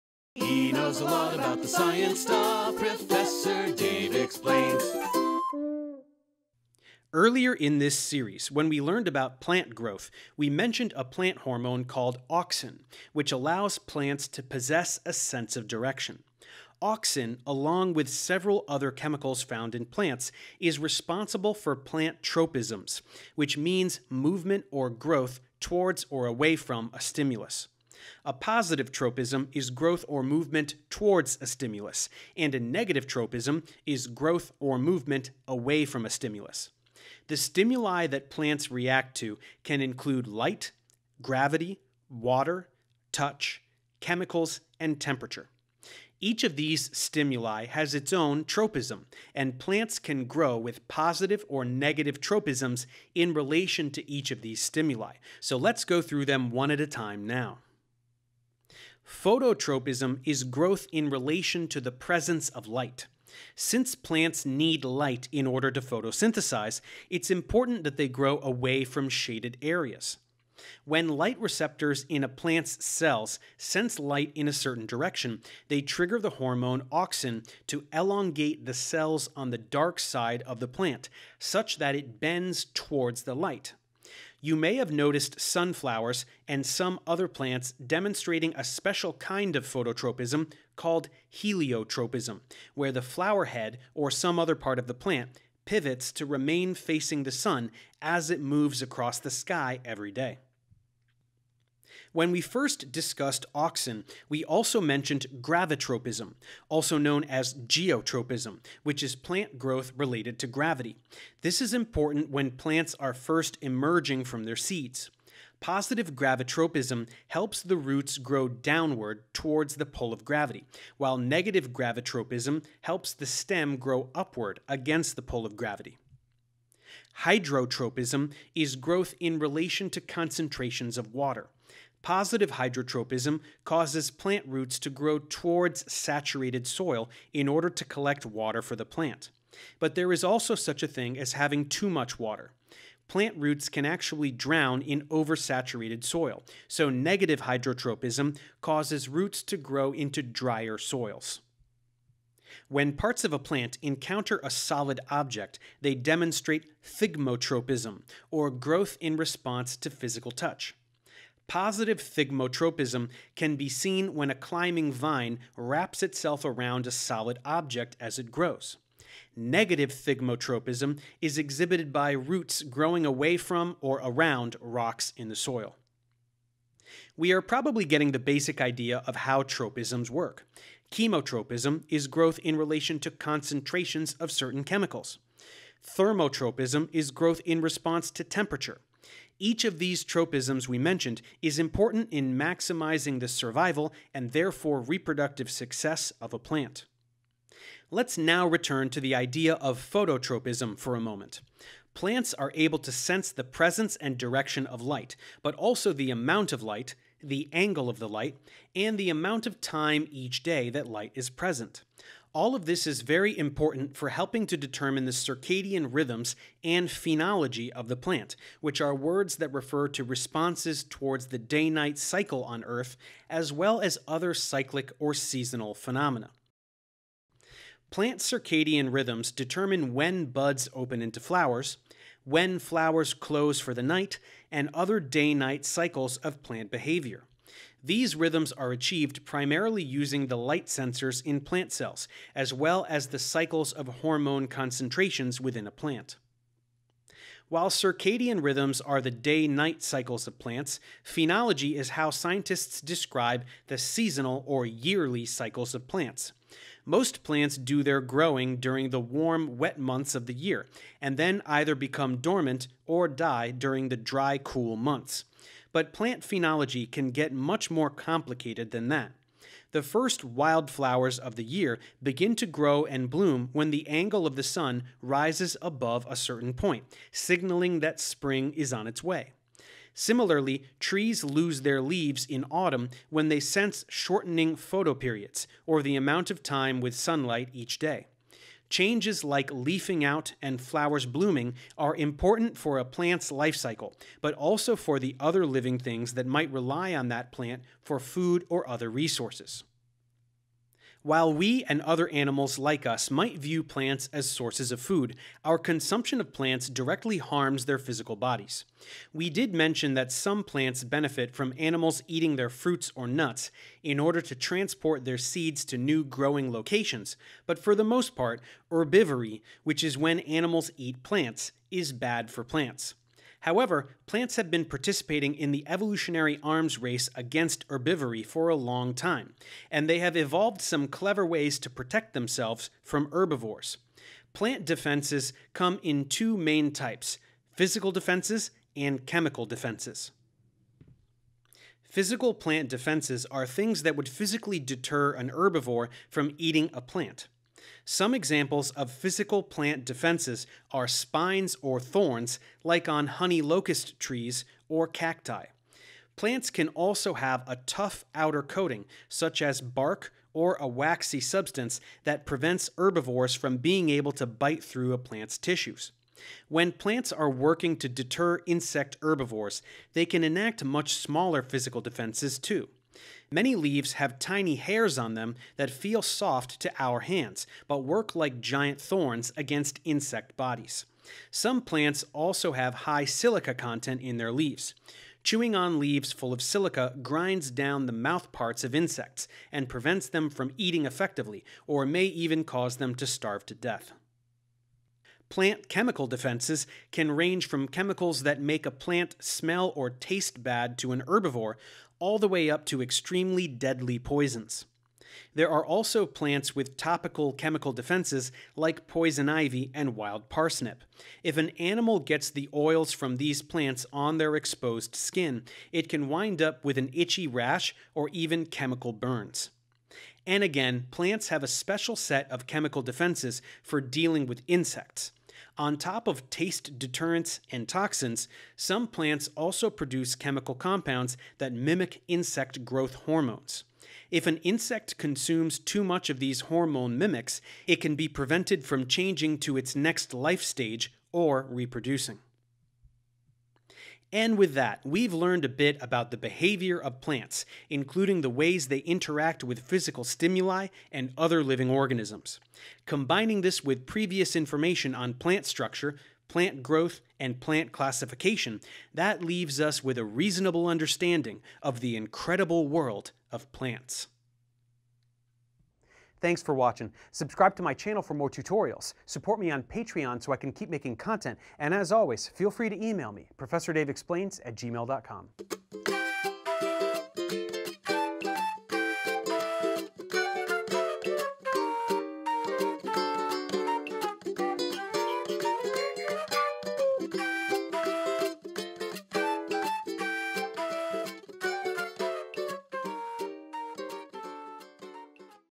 Plant Responses to the Environment: Tropisms and Defenses
We've already learned a little bit about plant hormones, but let's dig a little deeper so that we can really examine the different ways that plants can respond to stimuli. These are called tropisms, and they can be positive or negative, depending on whether plants grow or move towards or away from things like light and water. While we are on the subject, let's learn some of the ways that plants defend themselves against other organisms that would like to make a meal of them!

Script by Sheryl Hosler